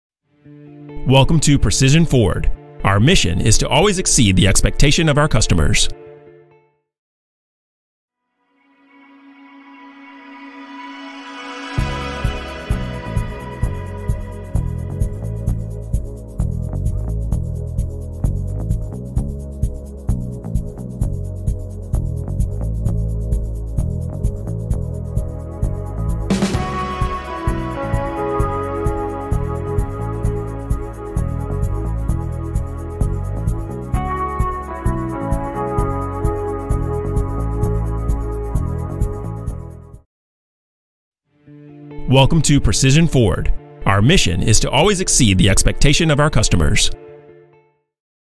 USED 2019 DODGE GRAND CARAVAN SXT at Precision Ford (USED)
This is a USED 2019 DODGE GRAND CARAVAN SXT offered in Lillington North Carolina by Precision Ford (USED) located at 945 North Main Street, Lillington, North Carolina

Stock Number: F4476